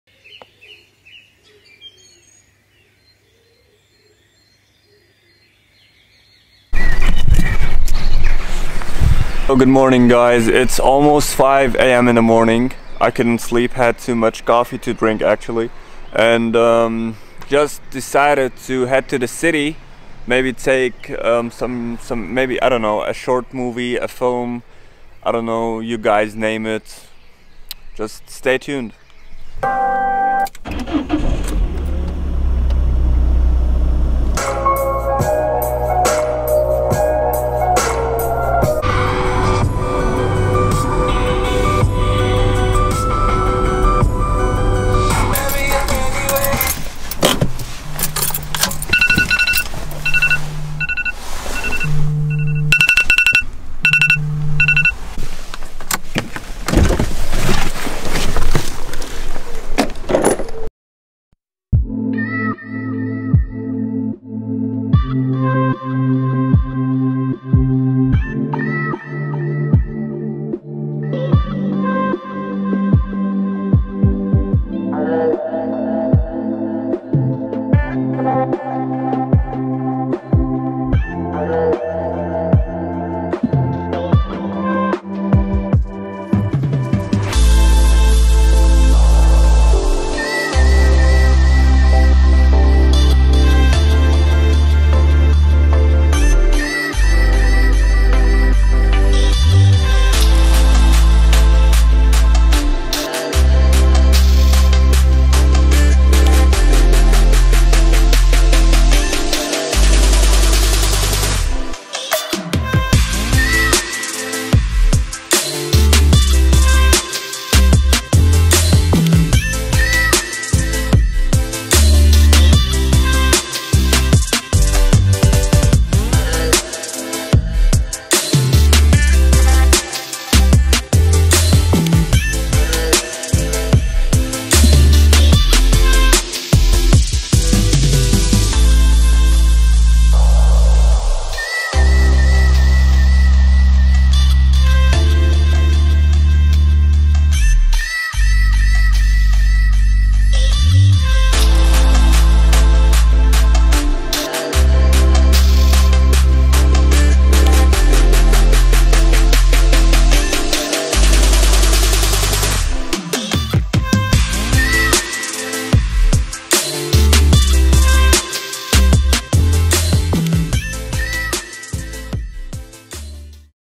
GOOD MORNING GERMANY !
i was wide awake today so i went to the city to explore more places to film
turns out münster is quite a beautiful city even if its a ghost town
i hope y'all enjoy the video and stay safe !
equipment and gadgets used :
Dji Ronin-sc
Røde microphone